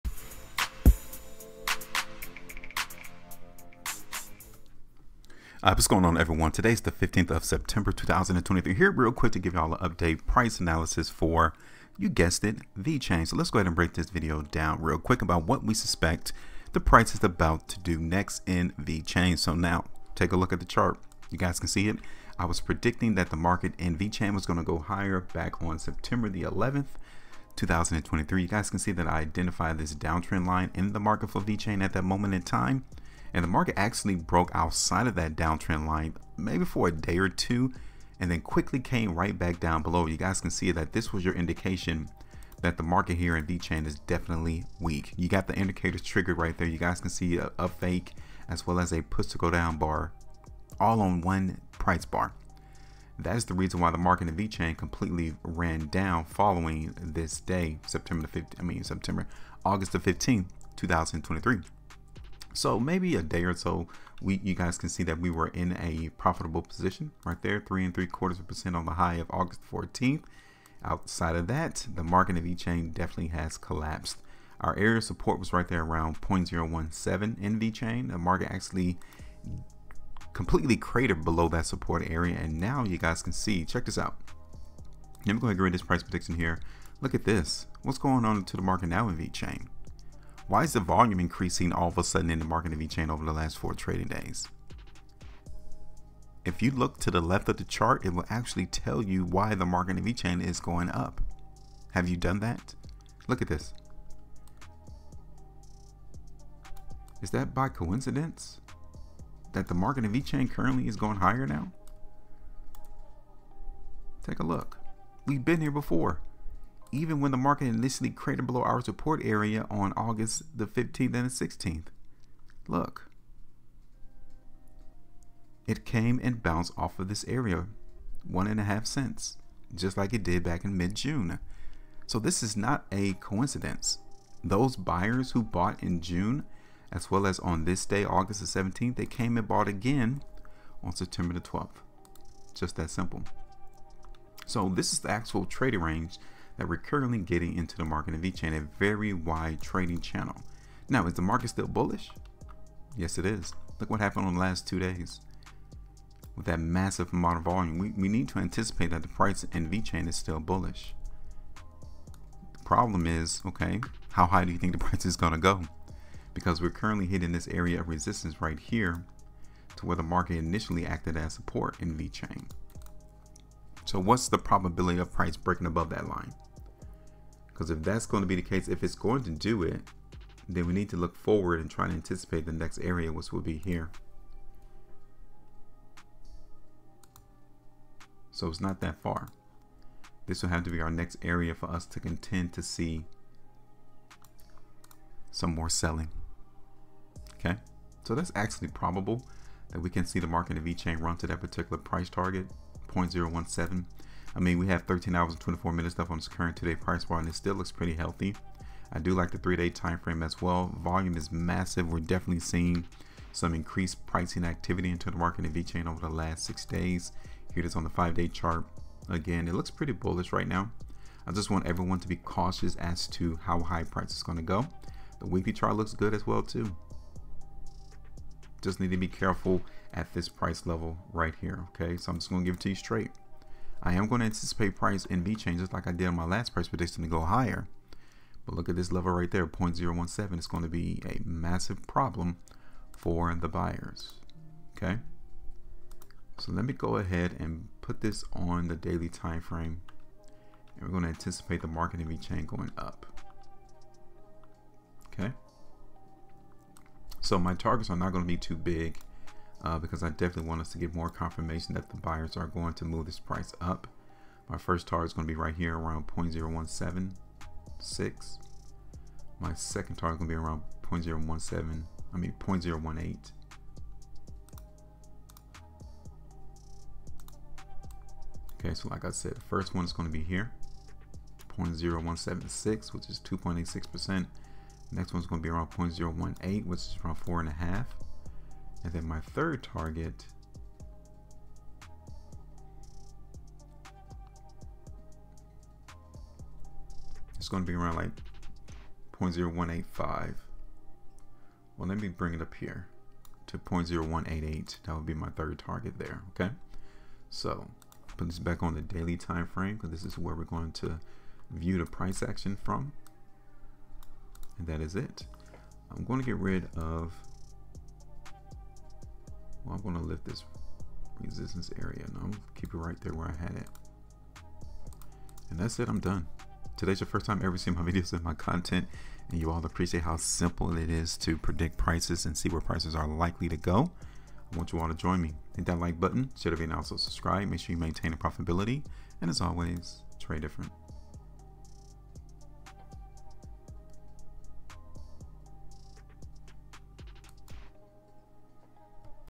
VeChain Price Analysis Showing Potential For Big VET Crypto Gains? VET Coin Price News
Is the Vechain price chart showing a potential for a big VET crypto price gains?